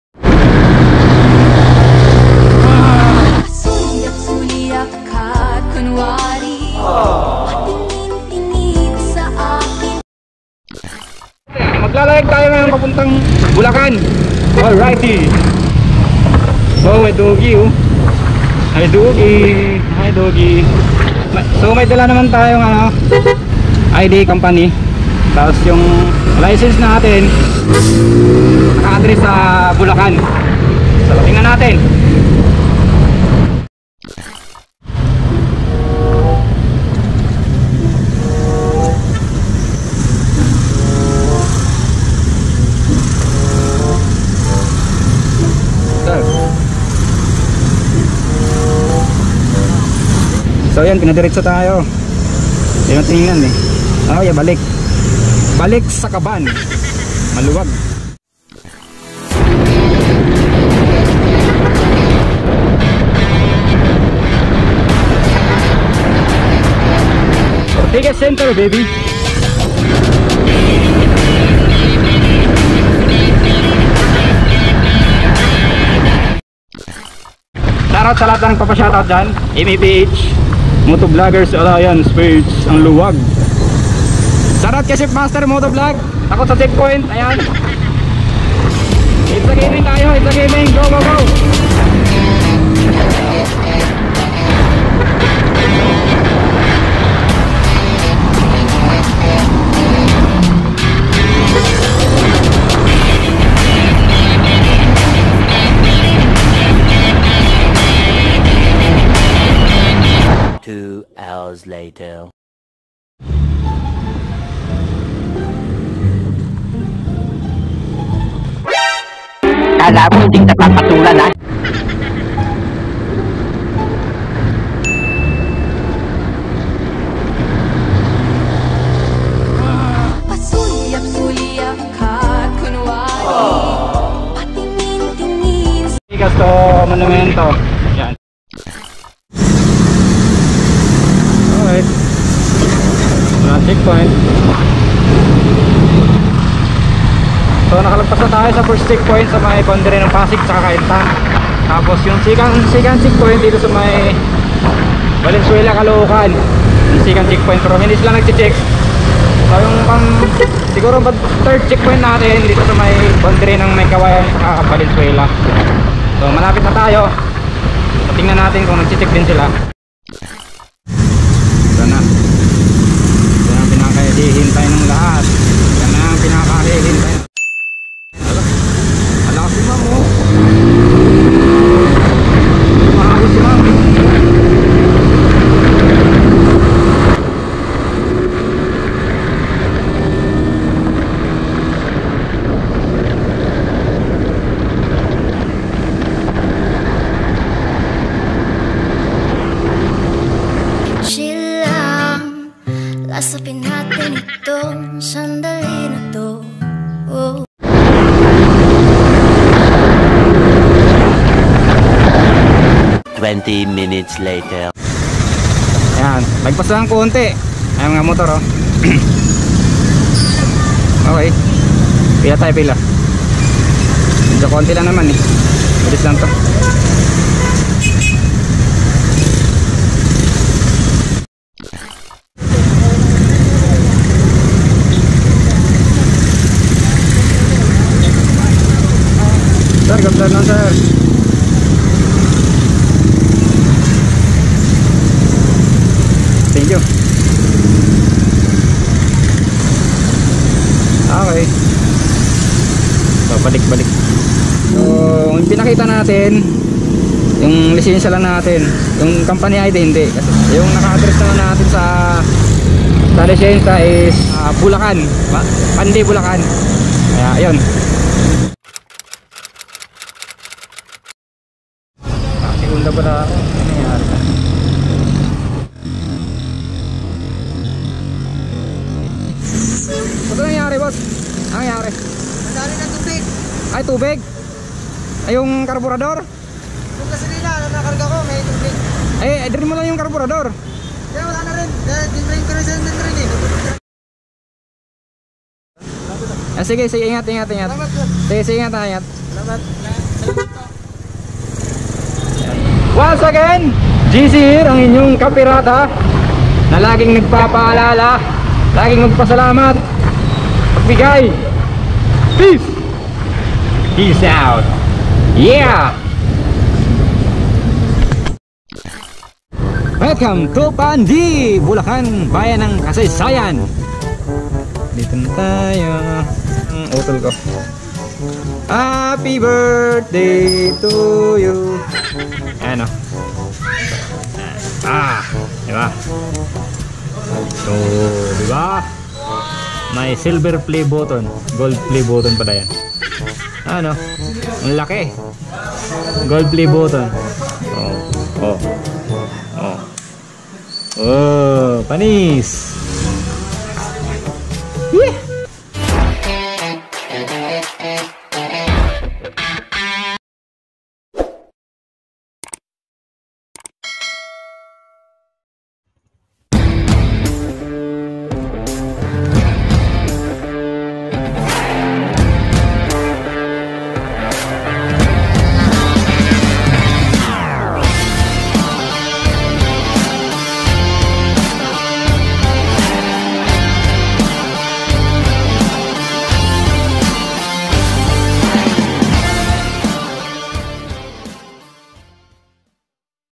MECQ,ECQ & GCQ CHECKPOINTS |LUSOT|JC MotoVLog
VLog title:MECQ,ECQ & GCQ CHECKPOINTS LUSOT RizaLtoBulacan Apor BilisNgByahe |JC MotoVLog

Buti NakaLusot Ang inyong Lingkod Nah Si KaH'Pirata. Basta Kumpleto Lng Sa Dokumento
Makakapasok Kayo..KeepSafe ALways Sa Inyo.
MagdaSaL Tayo Nah Nawa'y Matapos Nato.Amen

Intro Credits:Boss TL. PH

FbPage: JC MotoVLog
TiktokAccount:johnjeggercarrill16